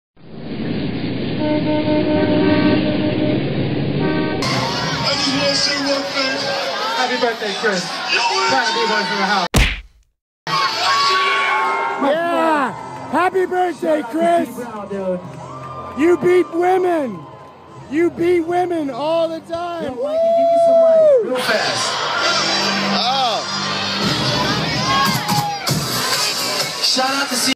Happy Birthday Chris Brown, You Beat Women!
This past Saturday night I was hanging out with some friends at Beachers Madhouse in the Roosevelt Hotel and Chris Brown showed up to celebrate his birthday. I decided to let him know what I thought of him.